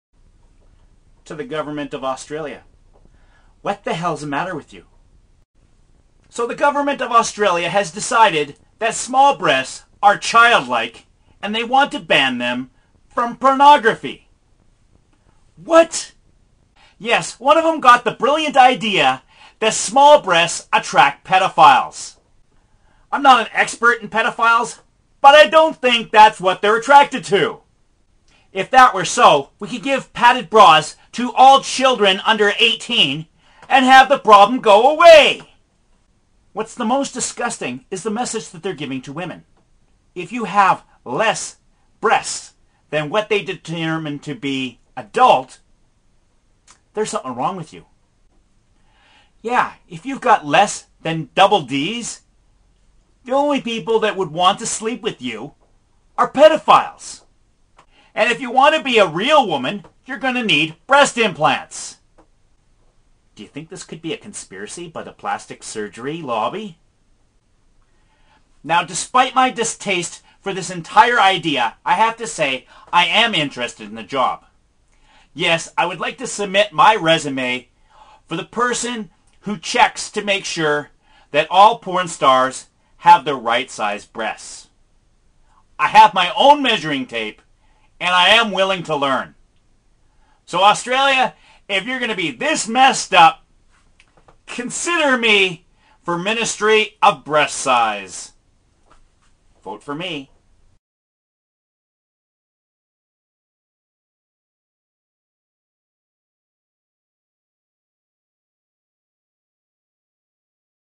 Austrailia Hates Small Breasts
Australia bans small breasts from porn because it encourages pedophilia. There is no science to back this claim up. Pedophiles are interested in children, not adults with small chests. Is Australia saying that women with small breasts are deformed? Less Adult? Should all women get breast implants to become adults? If a girl of 12 develops large breasts is she now considered an adult in Australia? Who decides what size of breasts is too small? Who measures the porn stars to make sure they 'measure up'?